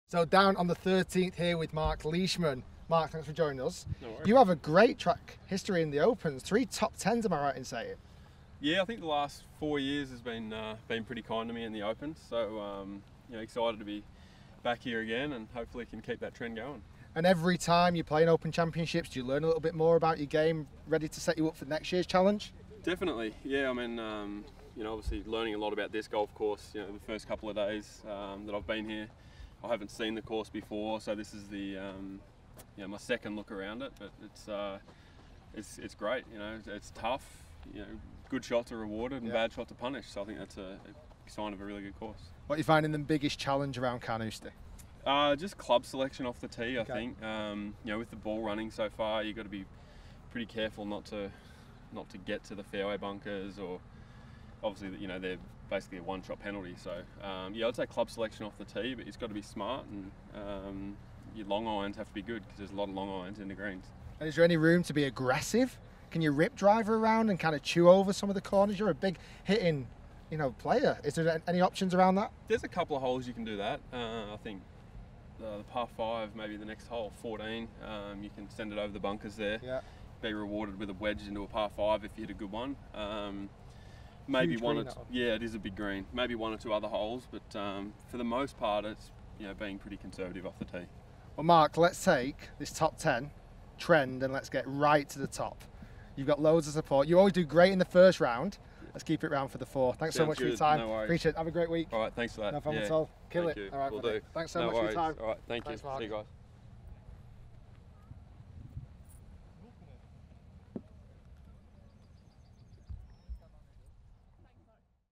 Rick Shiels walking 13 with Marc Leishman
Rick catches up with Marc Leishman to chat about his excellent Open record and his thoughts on a fiery Carnoustie.